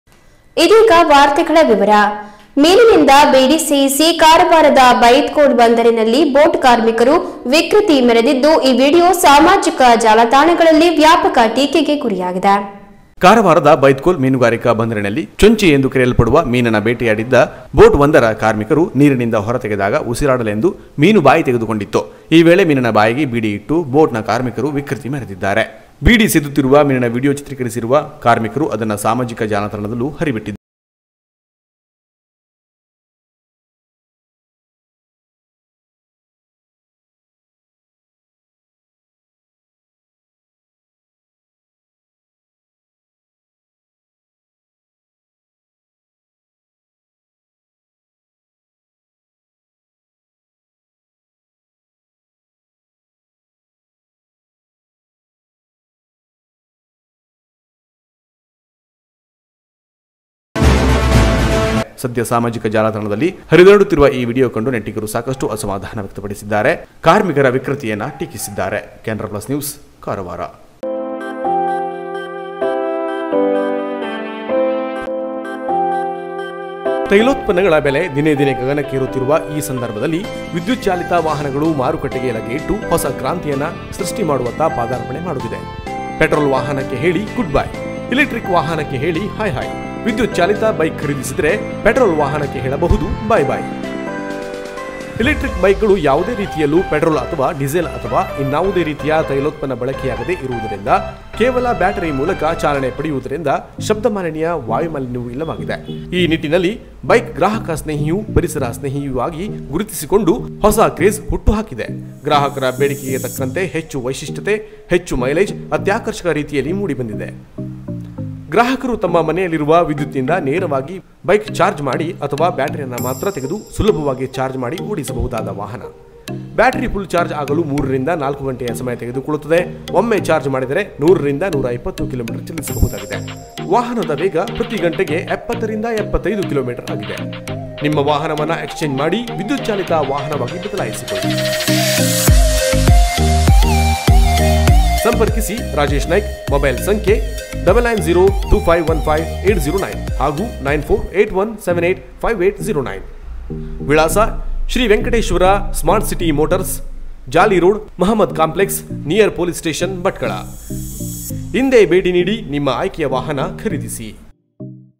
Canara Plus News Karwar Fish Smoking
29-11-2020 News ಮೀನಿನಿಂದ ಬೀಡಿ ಸೇಯಿಸಿ ಕಾರವಾರದ ಬೈತಖೋಲ್ ಬಂದರಿನಲ್ಲಿ ಬೋಟ್ ಕಾರ್ಮಿಕರು ವಿಕೃತಿ ಮೆರೆದಿದ್ದು, ಈ ವಿಡಿಯೋ ಸಾಮಾಜಿಕ ಜಾಲತಾಣಗಳಲ್ಲಿ ವ್ಯಾಪಕ ಟೀಕೆಗೆ ಗುರಿಯಾಗಿದೆ.